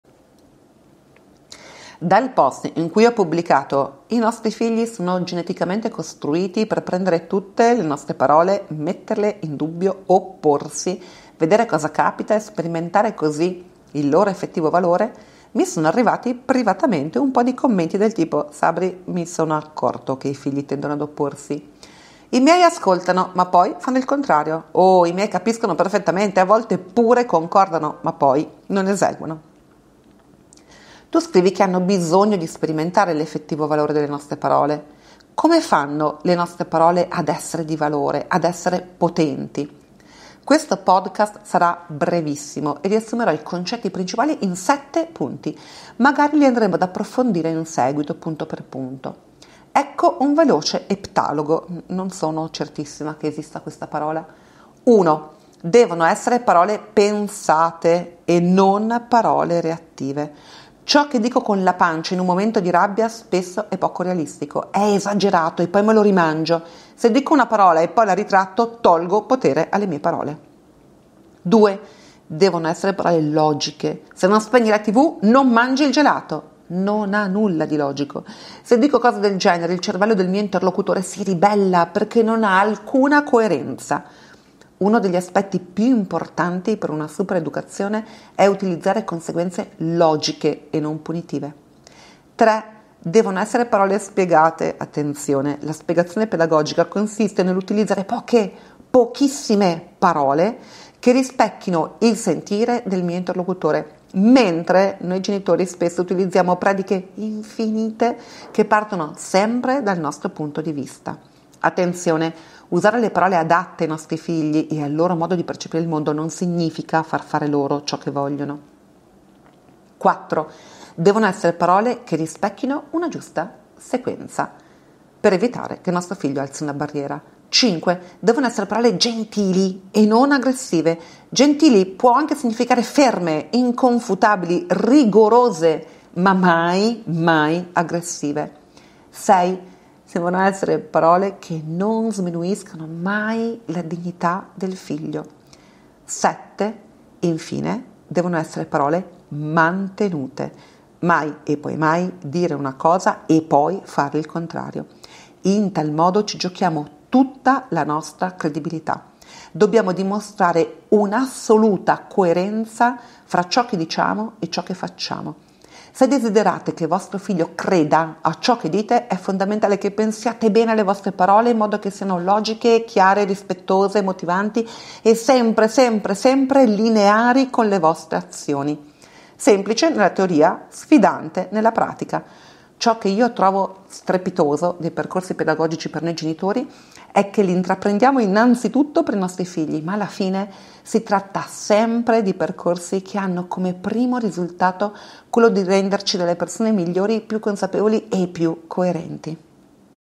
Cosa posso fare perché mio figlio mi creda?
7 modi per rendere le nostre parole potenti